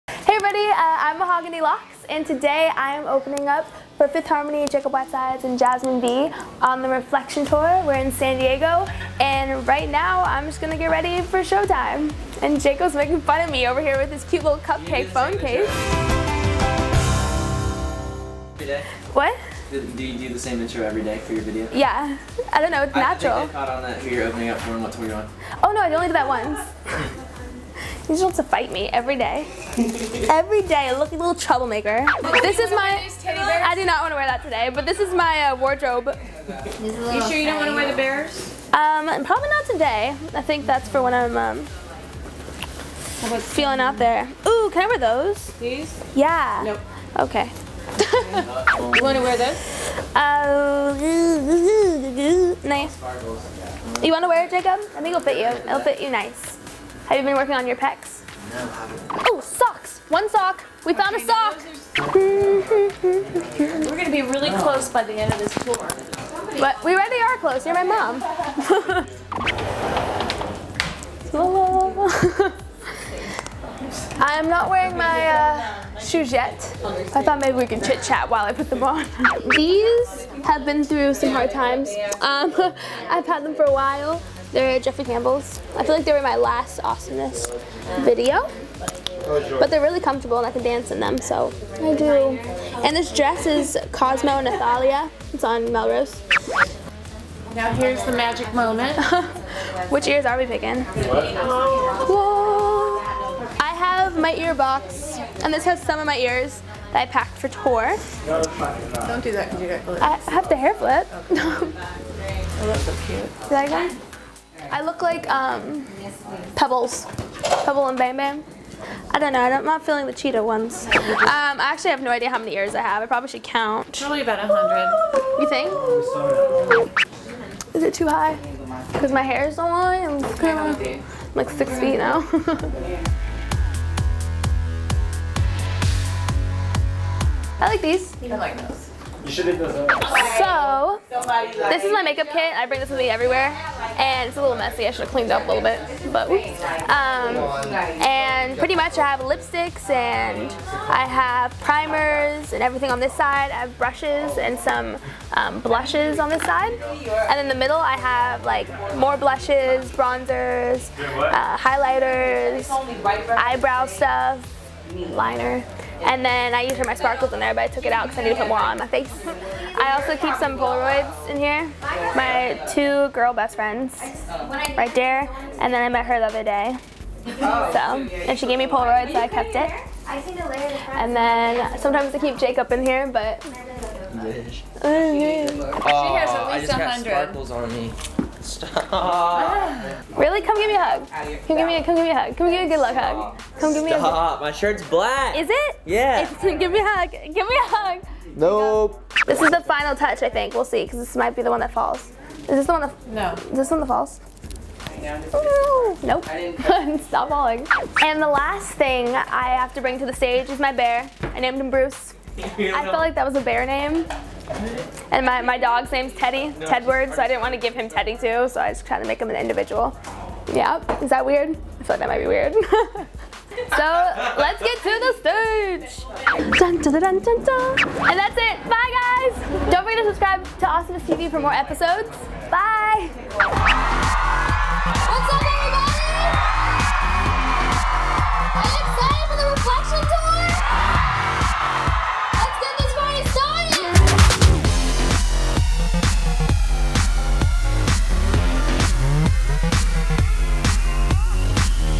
Mahogany LOX - Get Ready With Me - Mahogany Lox Takeover
Mahogany is opening on the reflection tour! She shows us how she gets ready for the show!!

→ Credits ←
Directed/Edited by: Ryan Bell

→ follow AwesomenessTV! ←